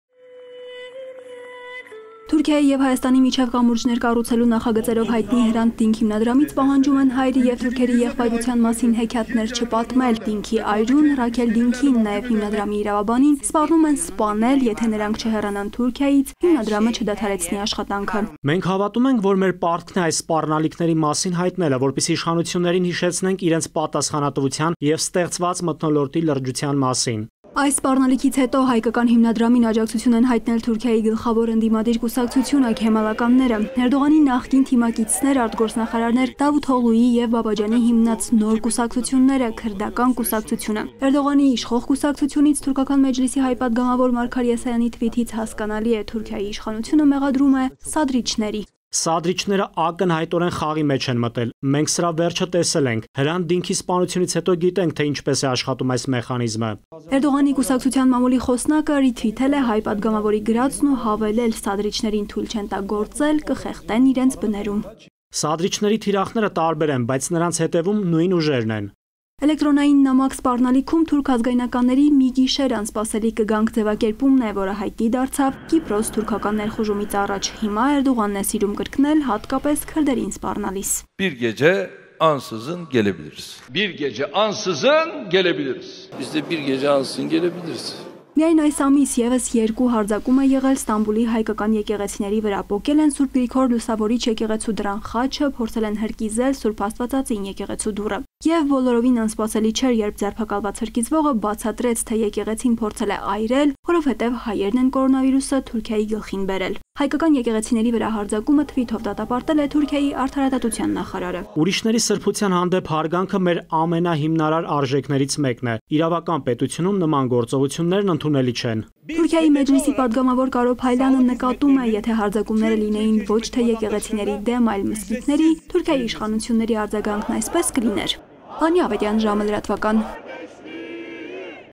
Թուրքիայում սպառնում են սպանել Հրանտ Դինքի այրուն
Թուրքիայի և Հայաստանի միջև կամուրջներ կառուցելու նախագծերով հայտնի«Հրանտ Դինք» հիմնադրամից պահանջում են հայերի և թուրքերի եղբայրության մասին հեքիաթներ չպատմել: Դինքի այրուն՝ Ռաքել Դինքին, նաև հիմնադրամի իրավաբանին սպառնում են սպանել, եթե նրանք չհեռանան Թուրքիայից, հիմադրամը չդադարեցնի աշխատանքը: